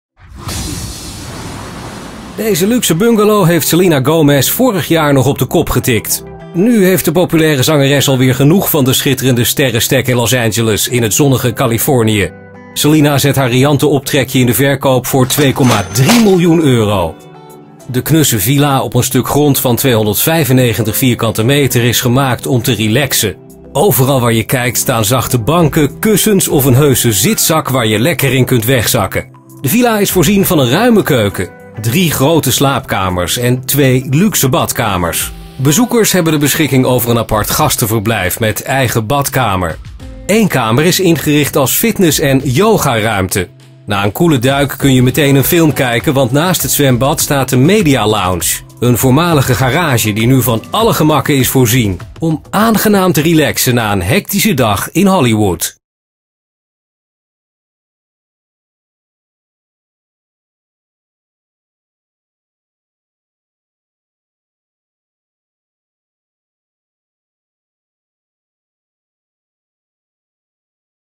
Selena Gomez verkoopt luxe Hollywood-villa
De zangeres heeft haar jaloersmakende optrekje in Californië te koop gezet voor 2,3 miljoen euro.